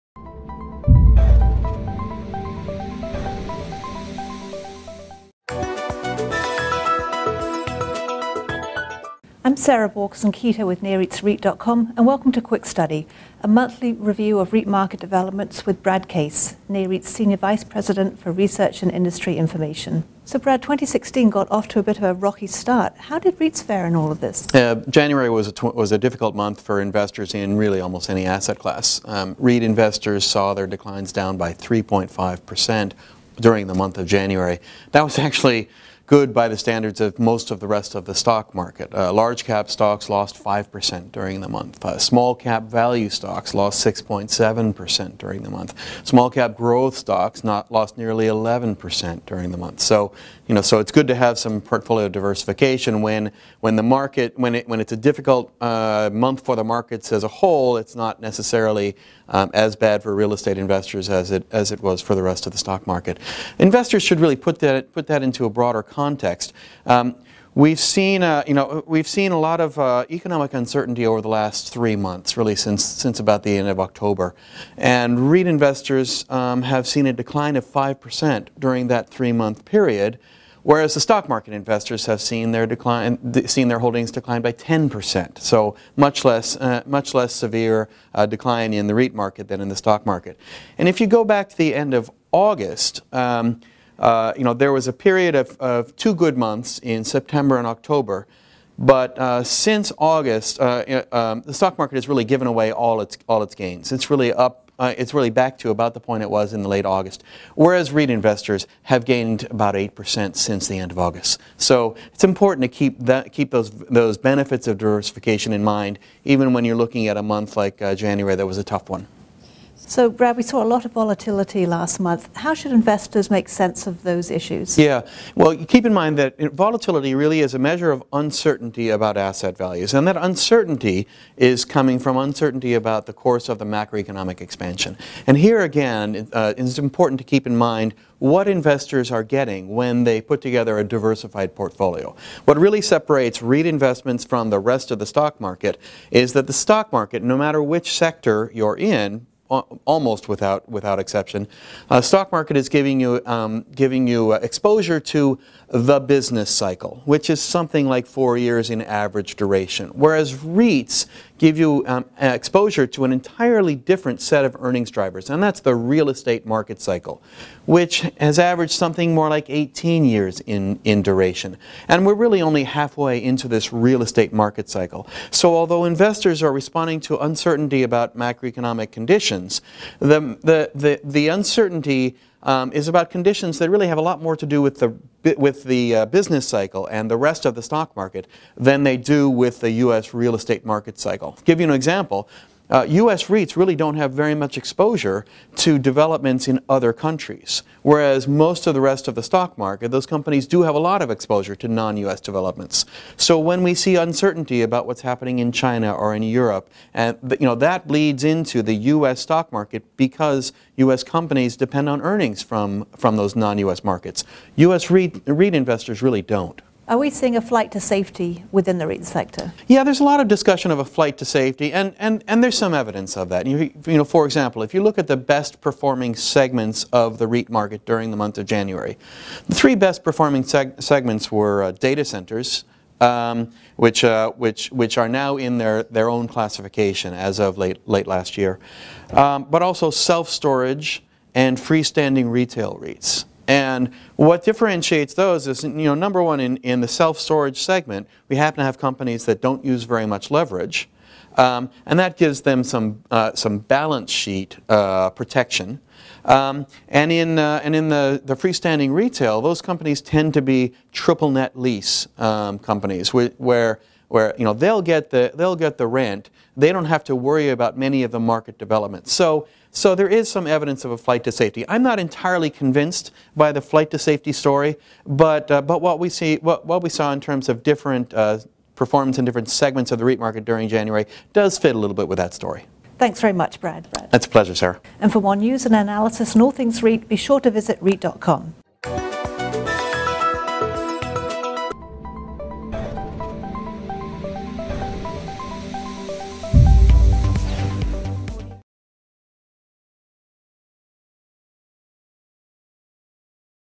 REITs Continue to Outpace Broader Market
In the latest edition of Quick Study, Brad Case, NAREIT’s senior vice president for research and industry information, pointed out that while REIT returns dropped modestly in February, the sector outperformed the broader market on a year-to-date basis.

The total returns of the FTSE/NAREIT All REIT Index fell 0.3 percent in February, while the S&P 500 Index lost 0.1 percent. For the year to March 3, the total returns of the FTSE/NAREIT All REIT Index dropped 0.1 percent, while the S&P 500 Index fell 2.1 percent.

“This is the kind of month where you don’t feel great, but it’s not a really bad month. It certainly helps to remind us of the importance of diversification because over the long term the REIT market and the rest of the stock market really do behave differently,” Case said.

Turning to REIT market trends in 2016, Case said he is seeing a growing awareness that assets are valued more highly in the private part of the real estate market than they are in the REIT sector.

“That’s curious,” Case observed. He explained that when information is released that impacts property prices, it shows up accurately in the REIT market. Values in the private market, however, are measured by appraisals or by infrequent transactions. As a result, it is much easier for valuations in the private market to become misaligned, Case stated. During the course of 2016, that tendency toward misvaluation may become evident as investment managers in the private part of the market look to purchase REIT-owned assets.

Meanwhile, the lodging sector was the REIT industry’s outperformer in February. Lodging REIT returns gained more than 7 percent. Case pointed out that after a long period of underperformance, “it may be that investors are seeing a way forward for the lodging sector that makes them much more optimistic.”

Returns in the manufactured homes segment were up more than 4 percent in February. Case stressed that manufactured homes have been a top performer for at least the last decade, reflecting the increasing professionalization of the industry.

3/4/2016 | By Sarah Borchersen-Keto